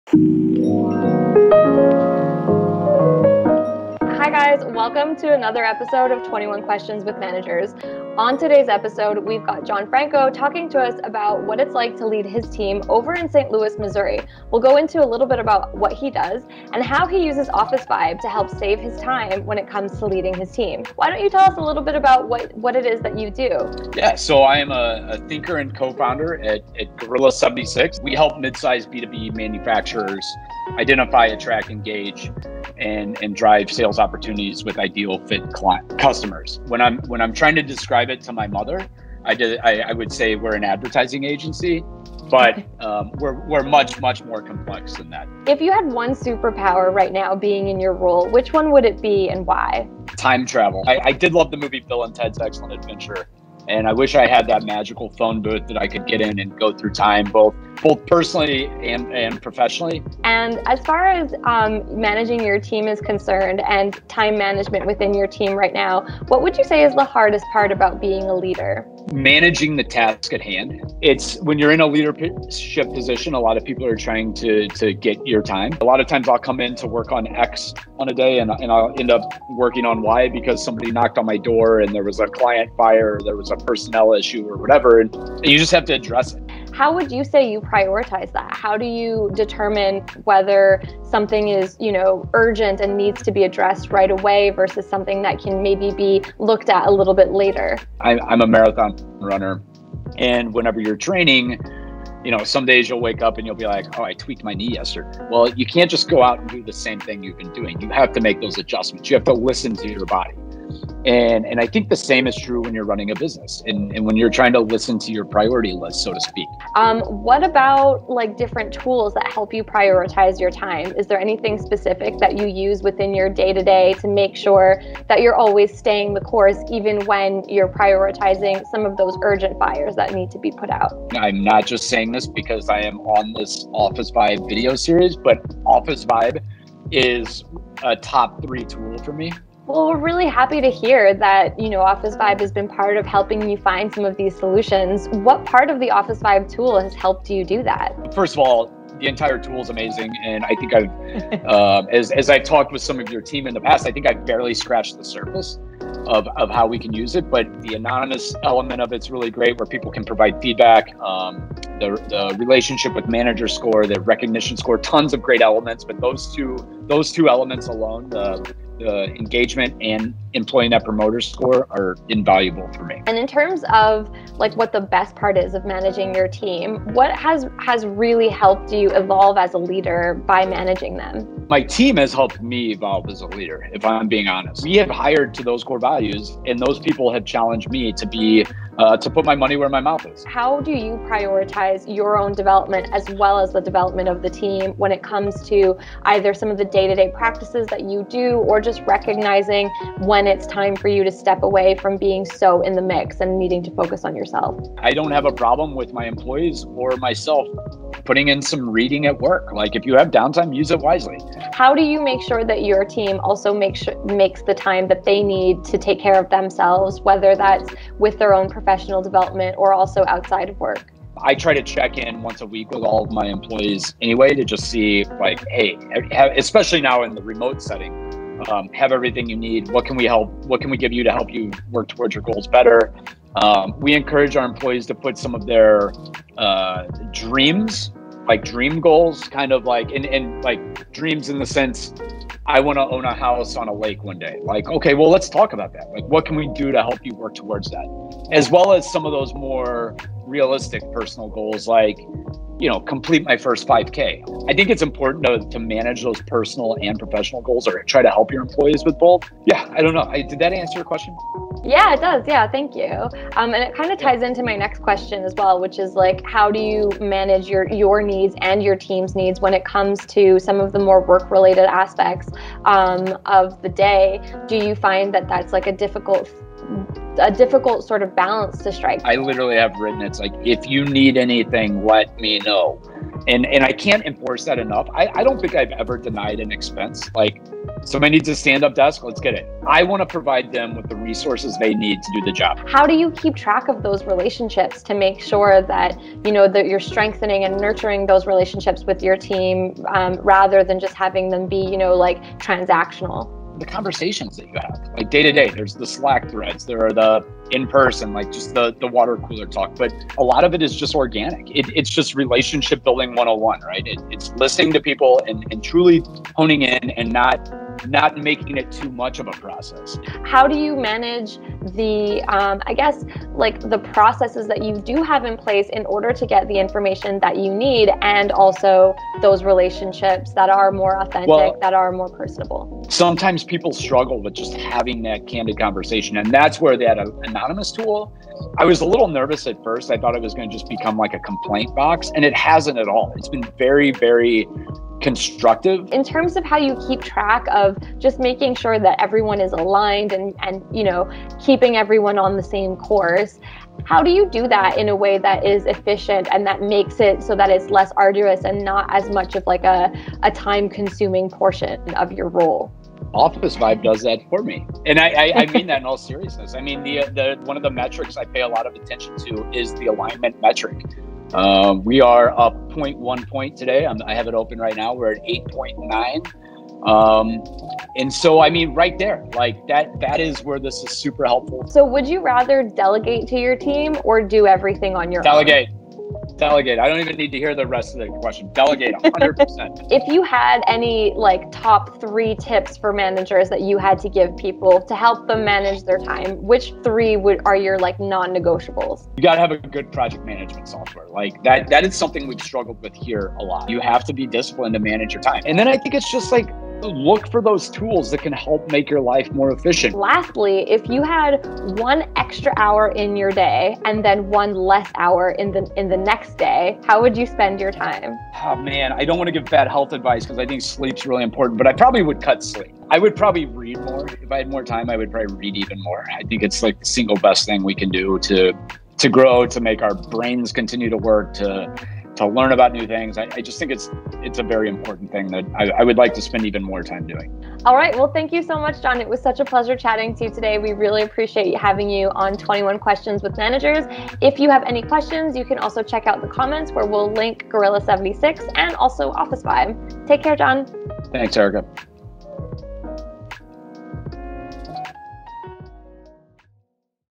How to better prioritize and manage your time– 21 Questions with Jon Franko
Time management can be tricky for managers who juggle many priorities. In this interview, Jon Franko walks us through how managers can be better at managing their time, and how Officevibe plays a key role to do it all.

How to better prioritize

Knowing what and how to prioritize is extremely important for managers to stay in control. You need to know where your focus and input is needed, really. Jon tells us some of the flags he pays attention to that help him know what to work on. And, unsurprisingly, how Officevibe makes that possible.

How to prioritize: 5 rules to be a more productive manager